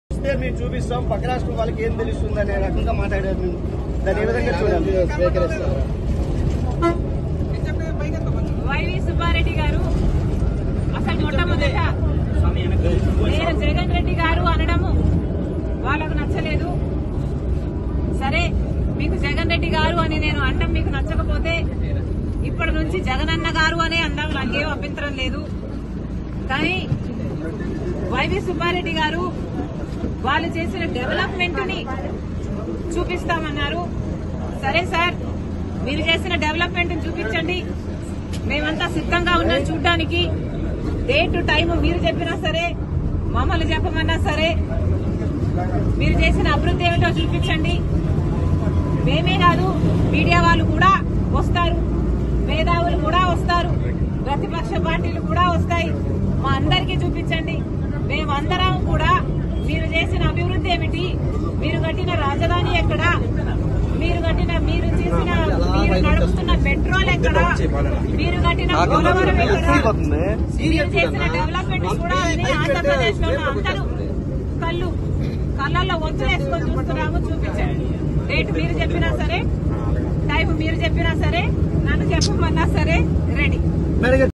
అభివృద్ధి పై జగనన్నకు సవాల్ విసిరిన ఏపీసీసీ షర్మిల.. డేట్ ఎప్పుడైన రెడీ..
SKLM 9 NEWS srikakulam CNG#